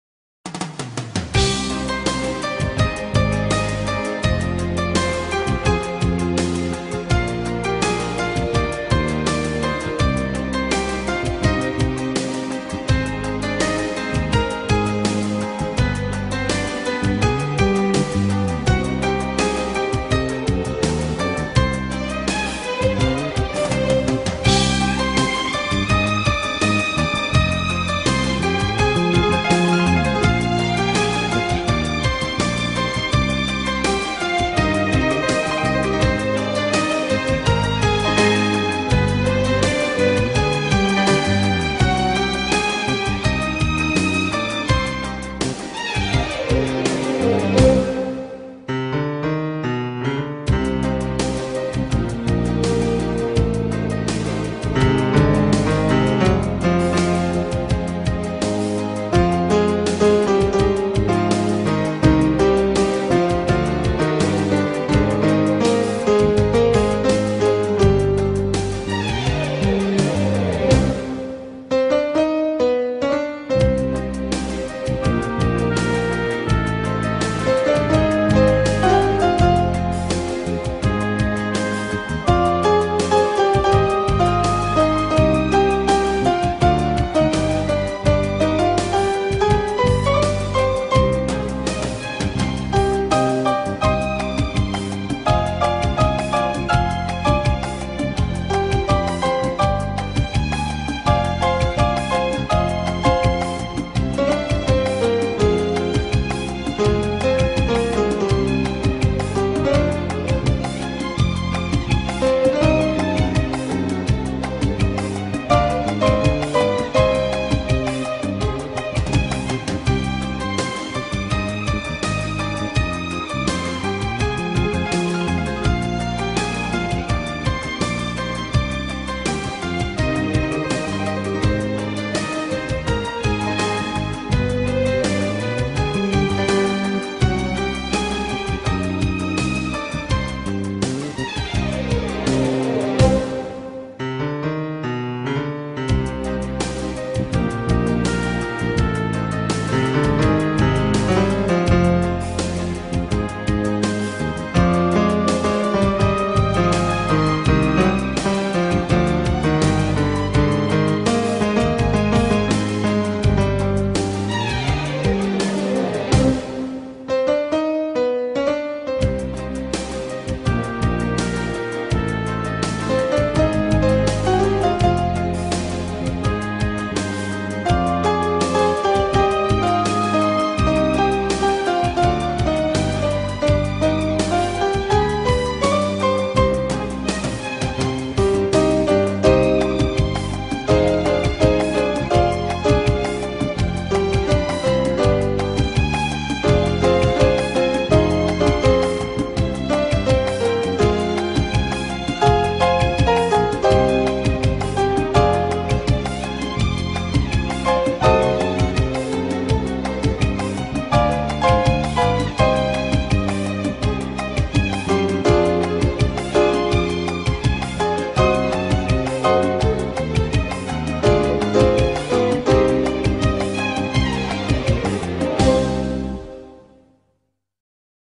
И ДУХНА ВЯТЪРА В ГЪРЛОТО...
Представям поредната история написана от Валентин Желязков /Napolitano/ която е токова истинска и преживяна от много ялюбени сърца.Всеки стих се забива в сърцето.